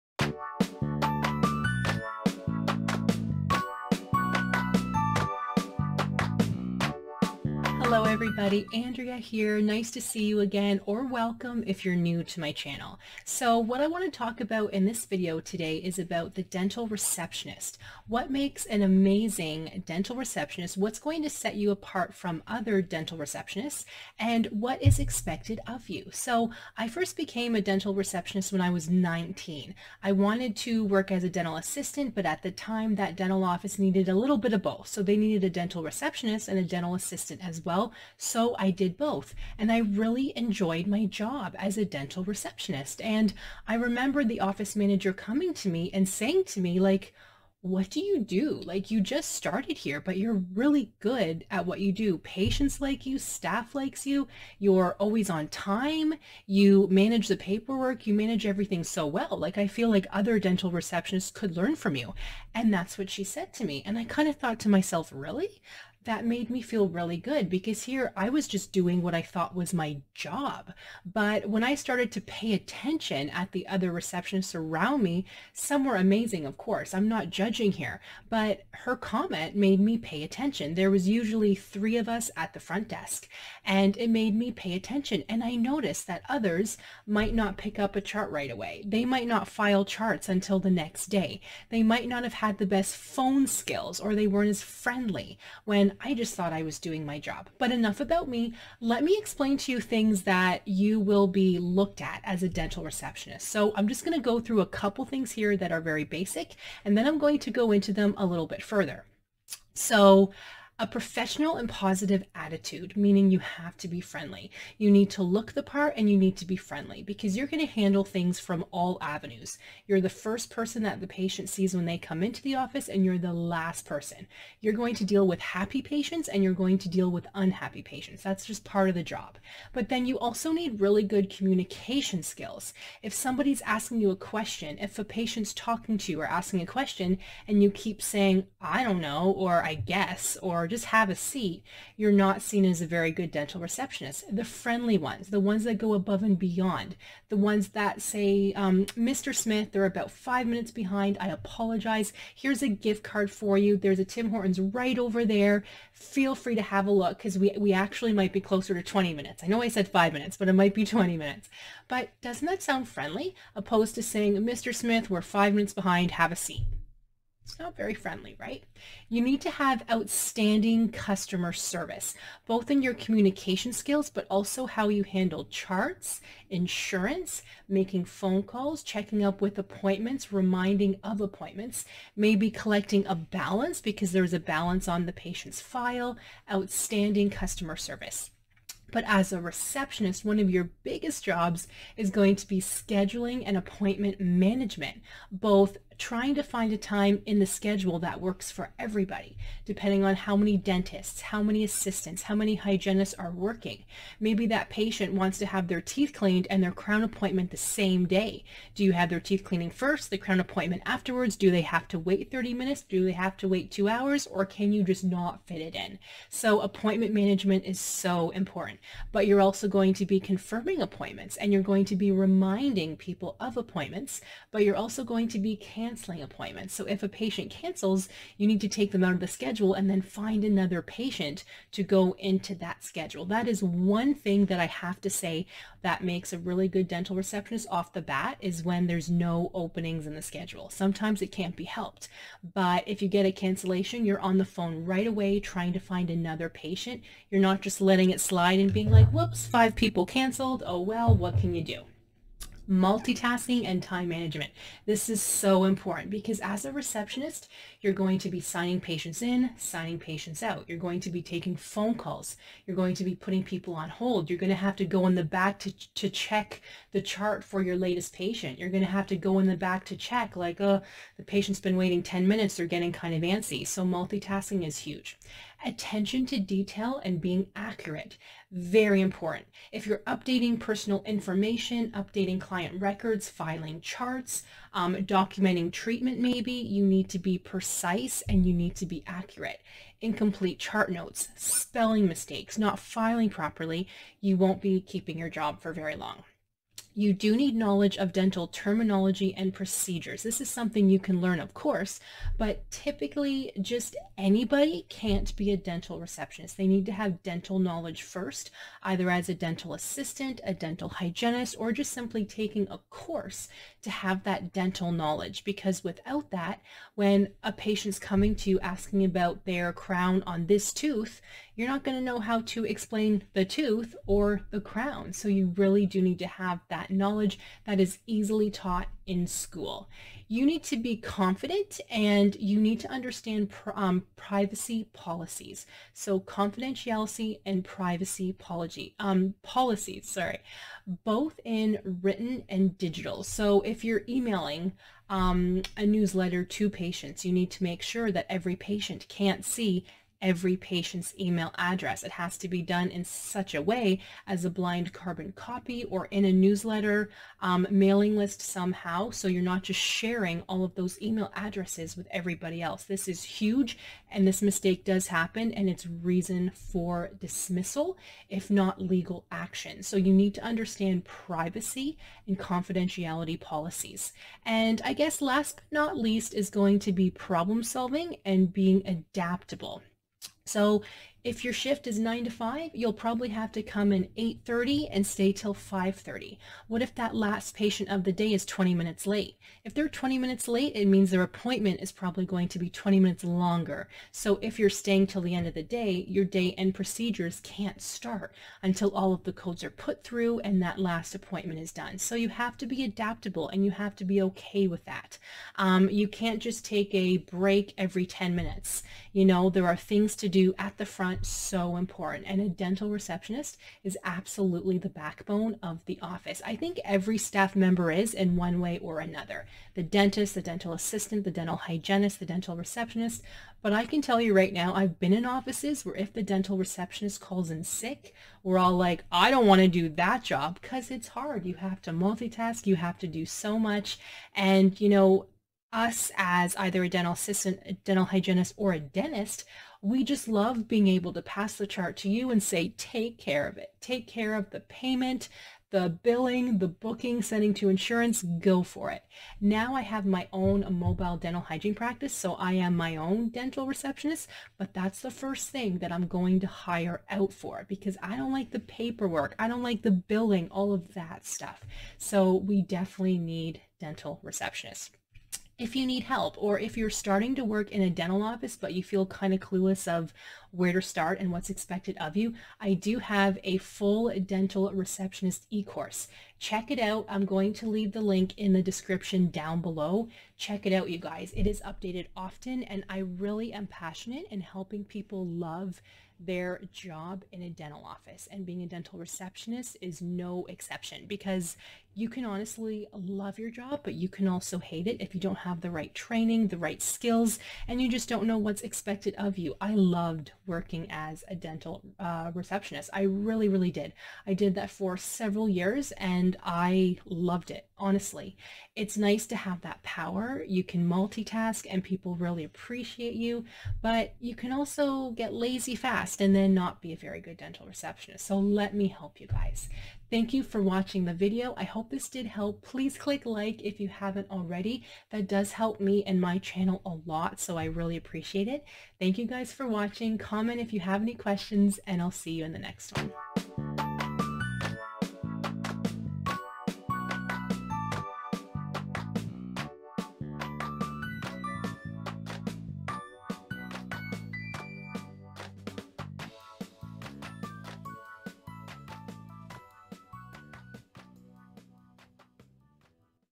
Dental Reception Beginners Course
Are you a new dental receptionist and have no idea where to start? I LOVED being a dental receptionist but I remember the overwhelm. This will get you 10 steps ahead!